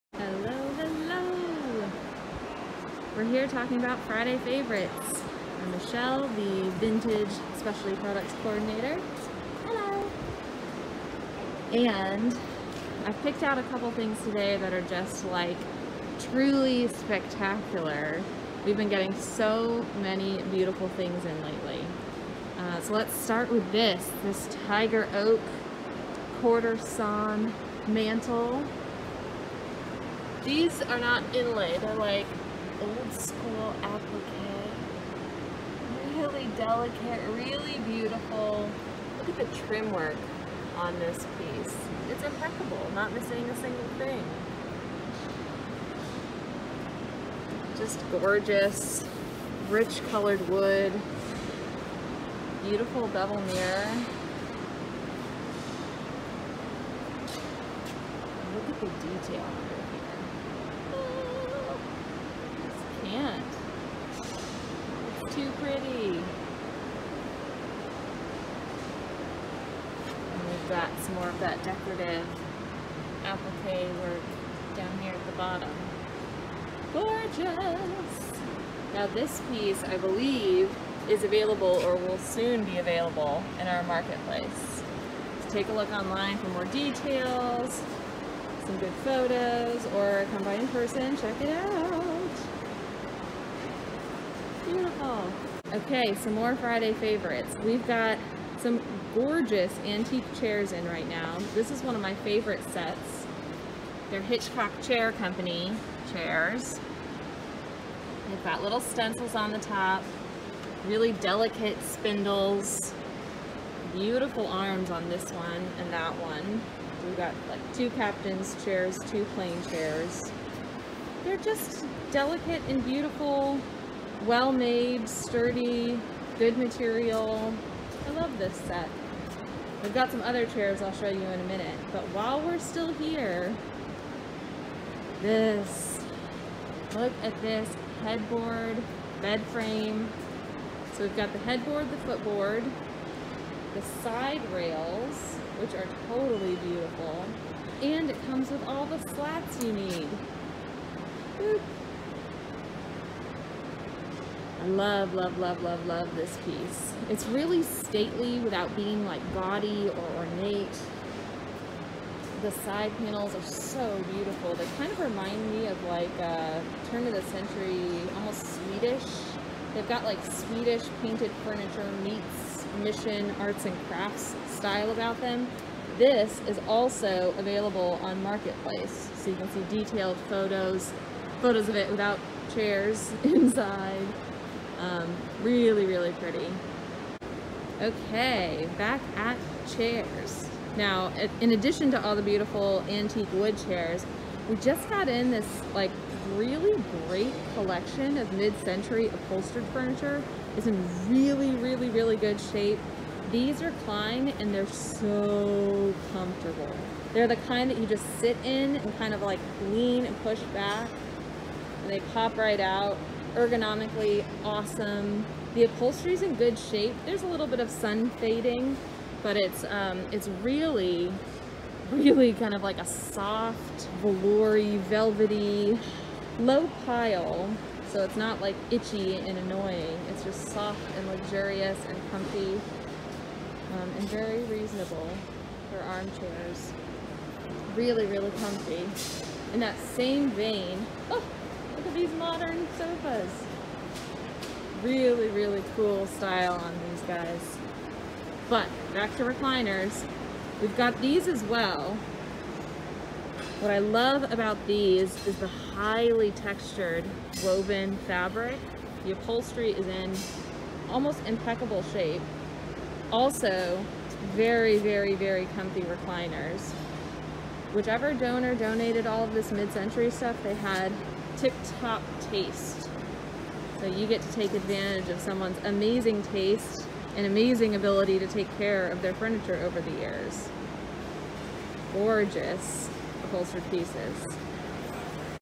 Friday Favorites: Fireplace Mantel and Chairs
In today’s tour of some of our vintage coordinator’s favorite vintage pieces in the reuse warehouse, check out an awesome tiger oak fireplace mantel, some beautiful mid-century upholstered recliners, and a set of vintage wooden chairs!